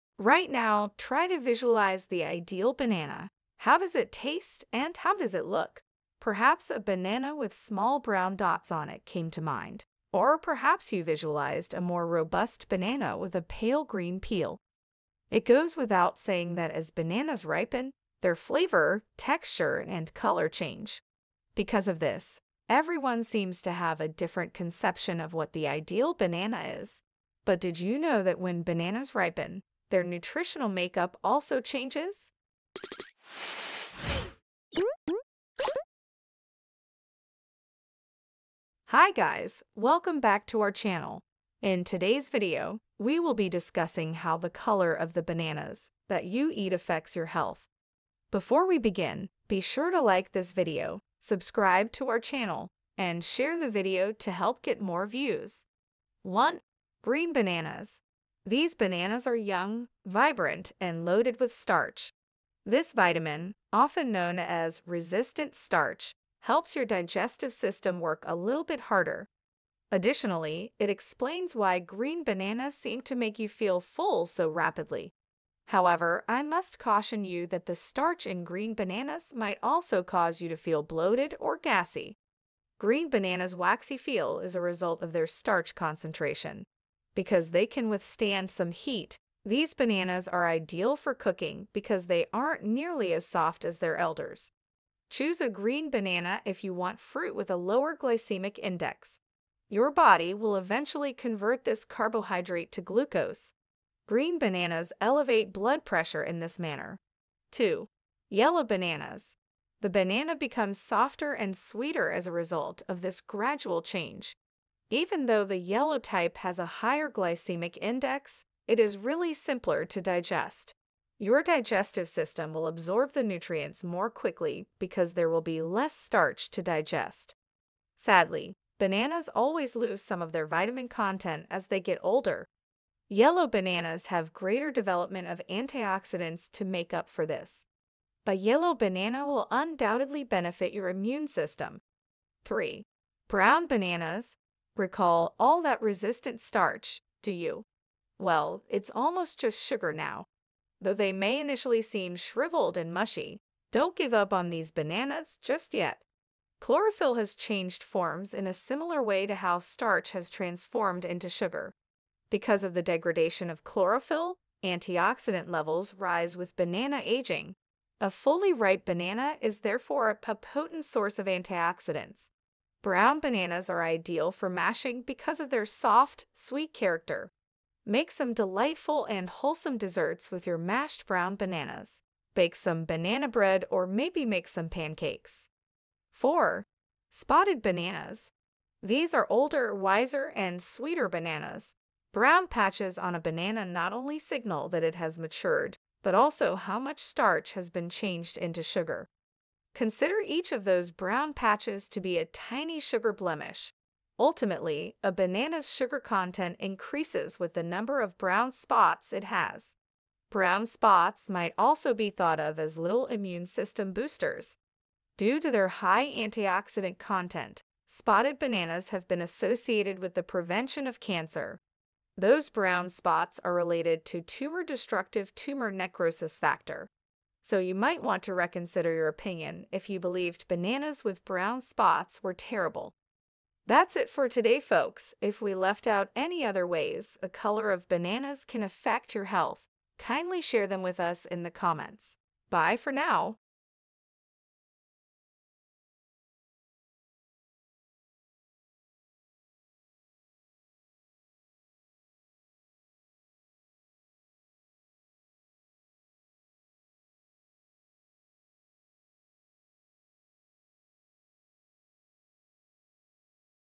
How The Color Of Bananas That You Eat Affects Your Health
Are You A Banana Lover? This Is How The Color Of Bananas That You Eat Affects Your Health

Right now, try to visualize the ideal banana. How does it taste and how does it look? Perhaps a banana with small brown dots on it came to mind. Or perhaps you visualized a more robust banana with a pale green peel.

It goes without saying that as bananas ripen, their flavor, texture, and color change. Because of this, everyone seems to have a different conception of what the ideal banana is. But did you know that when bananas ripen, their nutritional makeup also changes?

Which color banana is good for health?
How ripeness of a banana could affect your health?
Can bananas affect your health?
What are the effects of eating a green banana?

✅  Video Summary ✅

1. Green Bananas

These bananas are young, vibrant, and loaded with starch. This vitamin, often known as "resistant" starch, helps your digestive system work a little bit harder. Additionally, it explains why green bananas seem to make you feel full so rapidly. However, I must caution you that the starch in green bananas might also cause you to feel bloated or gassy. Green bananas' waxy feel is a result of their starch concentration. Because they can withstand some heat, these bananas are ideal for cooking because they aren't nearly as soft as their elders. Choose a green banana if you want fruit with a lower glycemic index. Your body will eventually convert this carbohydrate to glucose. Green bananas elevate blood pressure in this manner.

2. Yellow Bananas

The banana becomes softer and sweeter as a result of this gradual change. Even though the yellow type has a higher glycemic index, it is really simpler to digest. Your digestive system will absorb the nutrients more quickly because there will be less starch to digest. Sadly, bananas always lose some of their vitamin content as they get older. Yellow bananas have greater development of antioxidants to make up for this. A yellow banana will undoubtedly benefit your immune system.

3. Brown Bananas

Recall all that resistant starch, do you? Well, it's almost just sugar now. Though they may initially seem shriveled and mushy, don't give up on these bananas just yet. Chlorophyll has changed forms in a similar way to how starch has transformed into sugar. Because of the degradation of chlorophyll, antioxidant levels rise with banana aging. A fully ripe banana is therefore a potent source of antioxidants. Brown bananas are ideal for mashing because of their soft, sweet character. Make some delightful and wholesome desserts with your mashed brown bananas. Bake some banana bread or maybe make some pancakes.